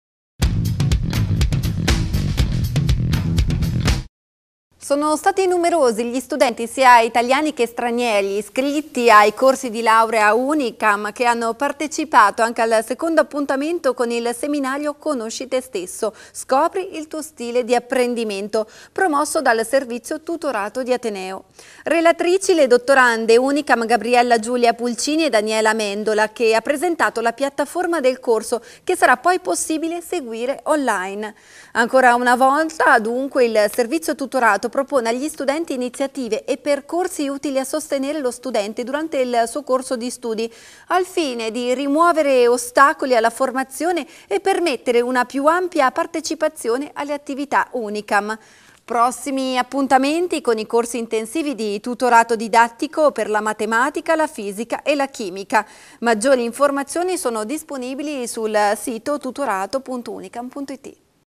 Seminari del Tutorato
Proseguono le iniziative del servizio tutorato
Sono stati numerosi gli studenti sia italiani che stranieri iscritti ai diversi corsi di laurea Unicam che hanno partecipato anche al secondo appuntamento con il seminario “Conosci te stesso. Scopri il tuo stile di apprendimento”, promosso dal servizio tutorato di Ateneo. Relatrici le dottorande Unciam Gabriella Giulia Pulcini e Daniela Amendola, che ha presentato la piattaforma del corso che sarà poi possibile seguire on line. Ancora una volta dunque il servizio tutorato propone agli studenti iniziative e percorsi utili a sostenere lo studente durante il corso di studi, al fine di rimuovere ostacoli alla formazione e permettere una più ampia partecipazione alle attività Unicam. Prossimi appuntamenti con i corsi intensivi di tutorato didattico per la matematica, la fisica e la chimica. Maggiori informazioni sono disponibili nel sito tutorato.unicam.it